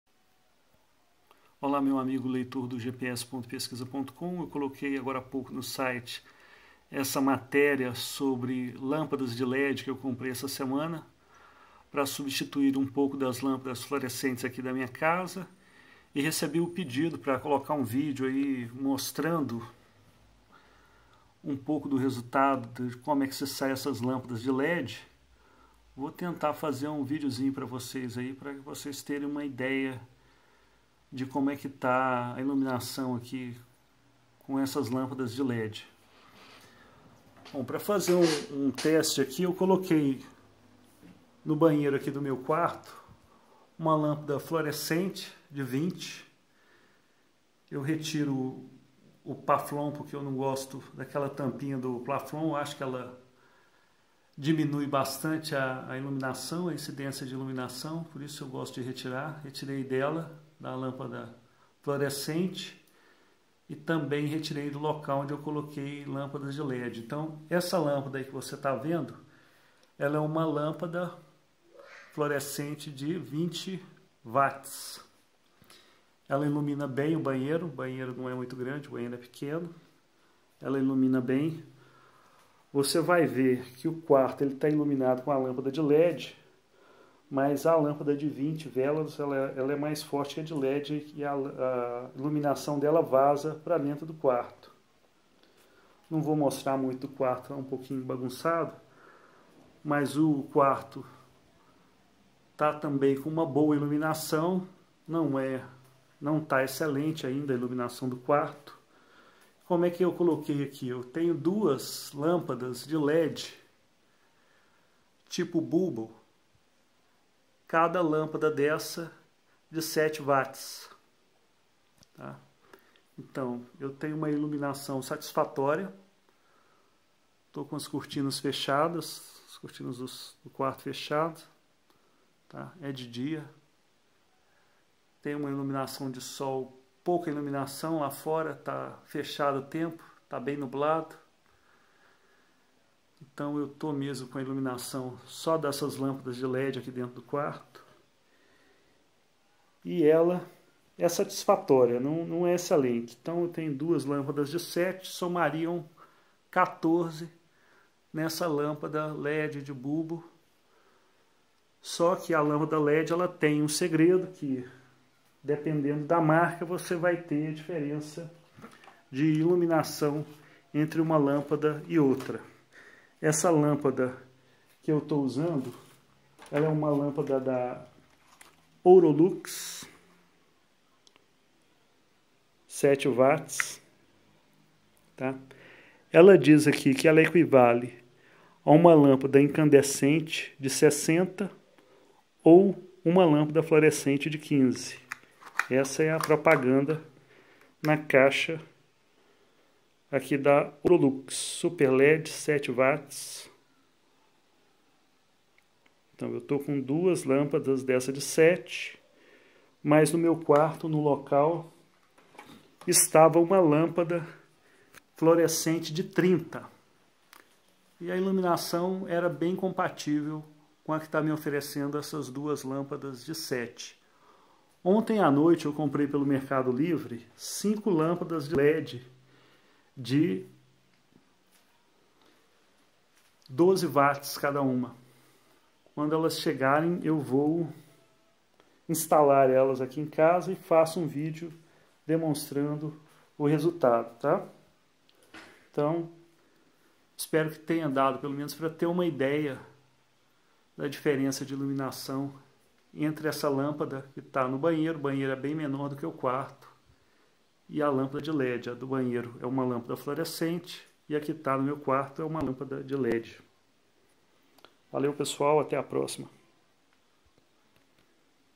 LÂMPADA DE LED COMPARADA COM LÂMPADA FLORESCENTE, INSTALADAS - 4K - GPS.Pezquiza.com
Pequeno teste de lâmpadas de Led para soquete E27 estilo bulbo das marcas Golden e Ourolux, comparação entre florescente e LED.

Canal do GPS.Pezquiza.com, acesse também o site para saber mais sobre Lâmpadas de LED e outros eletrônicos.

TRANSCRIÇÃO DESTE VÍDEO:

Olá amigo leitor do GPS.Pezquiza.com, eu coloquei agora a pouco no
site
essa matéria sobre a lâmpada de led que eu comprei essa semana
pra substituir um pouco das lâmpadas
florescentes aqui na minha casa
e recebi o pedido pra colocar um vídeo
um pouco do resultado de como é que se
sai essas lâmpadas de LED
vou tentar fazer um vídeo  pra
vocês terem uma idéia
de como é que tá a iluminação aqui
com essas lâmpadas de LED
Bom, pra fazer um
teste aqu eu coloquei
no banheiro de meu quarto
uma lâmpada florescente de 20
eu retiro
o paflon por que eu não gosto daquela
tampinha do paflon, eu acho que ela
diminuí bastante a iluminação, a incidência de iluminação, por isso gosto de tirar
retirei dela
da lâmpada
florescente
e também retirei do local onde eu coloquei lâmpada de LED, então essa lâmpada que
você está vendo
ela é uma lâmpada
florescente de 20
watts
ela ilumina bem o banheiro, o banheiro não é muito grande, o banheiro é pequeno
ela ilumina bem
você vai ver que o quarto ele está iluminado com a lâmpada de LED
mas a lâmpada de 20 velas ela é mais
forte do que a de LED
e a iluminação dela vaza para dentro do quarto
não vou mostrar muito o quarto não, ela tá um pouquinho bagunçado
mas o quarto
ele tá também com uma boa iluminação
não é
não tá excelente ainda a iluminação
do quarto
como é que eu coloquei aqui, eu tenho
duas lâmpadas de LED
tipo bulbo
cada lâmpada dessa
de sete watts
então eu tenho uma iluminação
satisfatória
tô com as cortinas fechadas
cortinas do quarto fechadas
tá até de dia
iluminaçãodo sol
pouca iluminação, lá fora tá fechado o tempo, tá bem nublado
então eu to mesmo com a iluminação só dessas lâmpadas de LED aqui do quarto
e ela
é satisfatória, não é excelente, então eu tenho duas lâmpadas de LED de sete
somariam
catorze
nessa lâmpada LED de bulbo
só que a lâmpada LED ela tem um segredo que
dependendo da marca você vai ter
diferença
de iluminação
entre uma lâmpada e outra
nessa lâmpada que
eu estou usando
é uma lâmpada
da Ourolux
7 Watts
ela diz aqui que ela equivale
a uma lâmpada incandescente de 60W
ou uma lâmpada florescente de 15W
essa é a propaganda
na caixa
aqui da Ourolux Super LED 7W
então eu to com duas lâmpadas dessa de 7
mas no meu quarto no local
estava uma lâmpada
florescente de 30
e à iluminação era bem compatível
com a que está me oferecendo essas duas lâmpadas de 7
ontem à noite eu comprei pelo mercadolivre
cinco lâmpadas led
de
12W cada uma
quando elas chegarem bem eu vou
instalar elas aqui em casa e faço um
vídeo
demonstrando
o resultado
espero que tenha dado pelo menos pra
ter uma idéia
na diferença de iluminação
entre essa lâmpada que tá no banheiro, o banheiro é bem menor do que o quarto
e a lâmpada de led, a do banheiro é uma lâmpada florescente
e a que tá no meu quarto é uma lâmpada de LED
valeu pessoal até a próxima